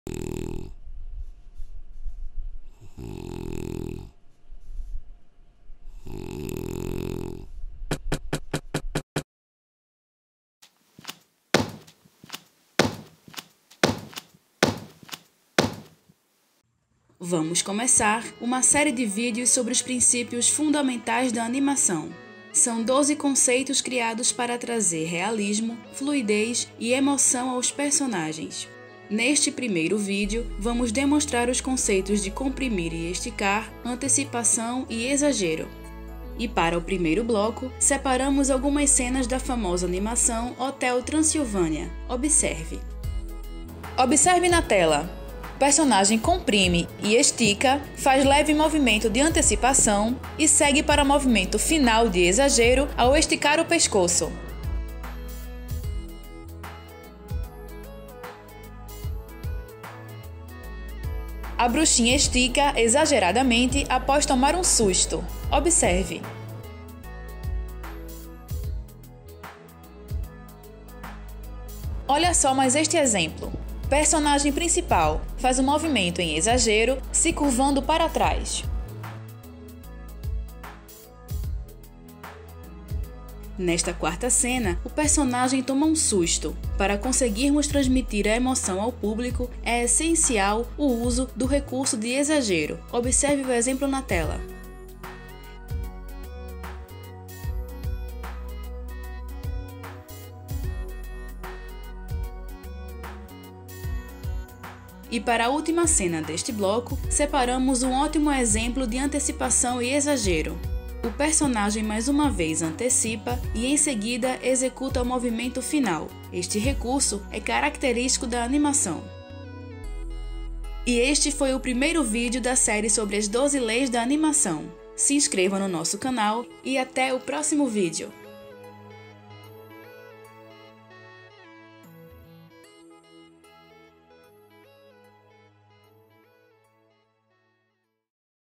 12 Leis da Animação - Exagero, esticar e encolher e antecipação.
Primeiro Video da Série das 12 Leis da Animação.
Neste vídeo explicamos do que se trata as leis de exagero, esticar e encolher e antecipação.

Edição e narração feita pelos estudantes Cristiano Albuquerque, Nilo Sandes e Raquel Ramos do curso de Computação Gráfica do IFPE Olinda.